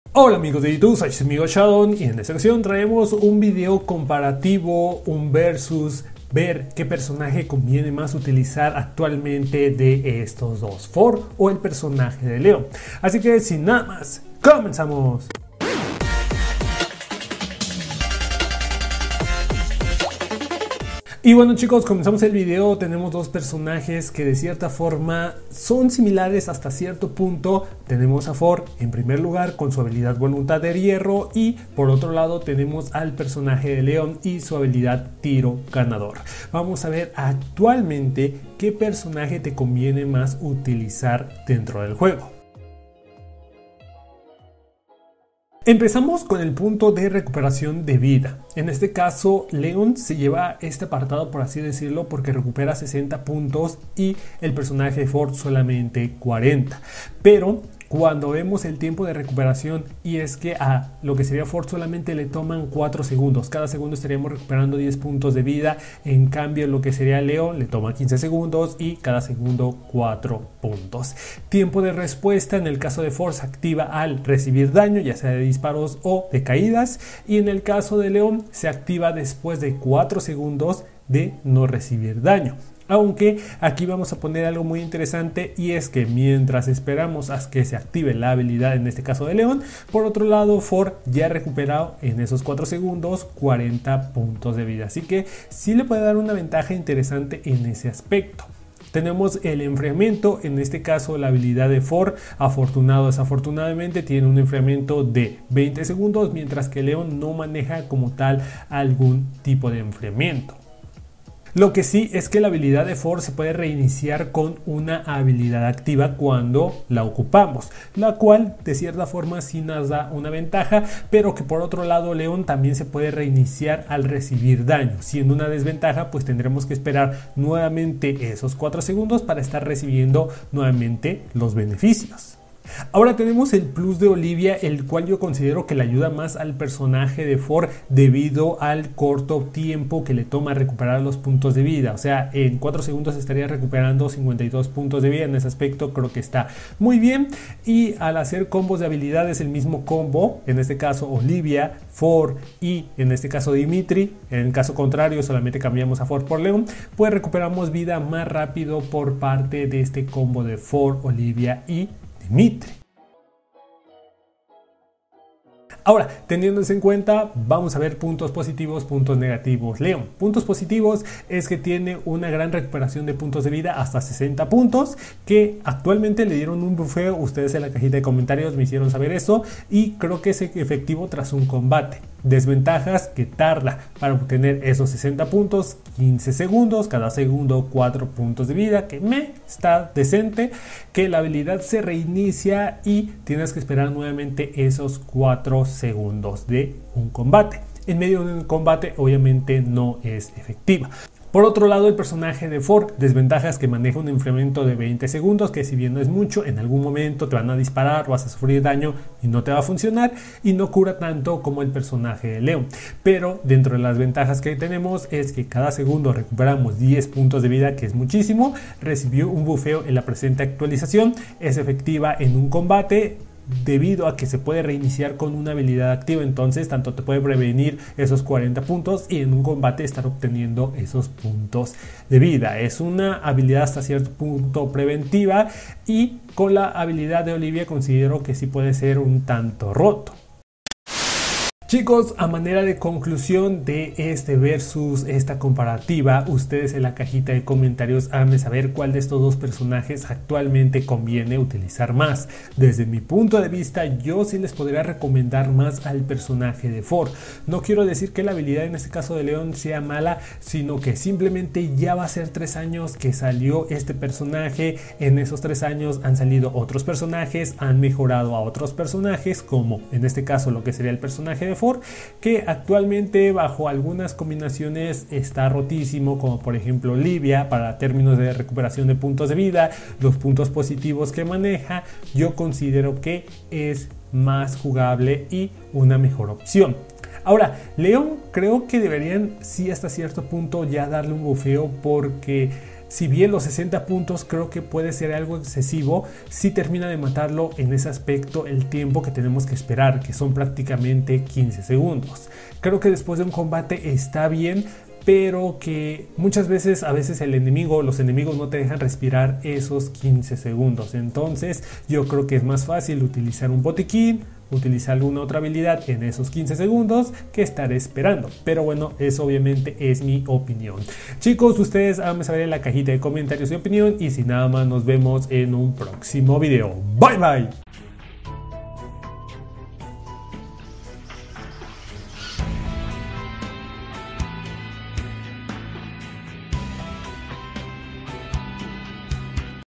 ¿ACTUALMENTE QUE PERSONAJE ES MEJOR USAR? - ¿FORD O LEON? | SHADOWGAMES
0:00 INICIO
0:21 CONTEXTO
0:48 RECUPERACIÓN DE VIDA
0:57 TIEMPO DE RECUPERACIÓN
1:09 TIEMPO DE RESPUESTA
1:58 ENFRIAMIENTO
1:51 REINICIO
2:12 +OLIVIA
2:26 ¿MÁS RÁPIDO DE CURAR?
2:43 VENTAJAS O DESVENTAJAS
2:46 LEON
3:21 LEÓN
4:04 ¿ES MEJOR USAR FORD?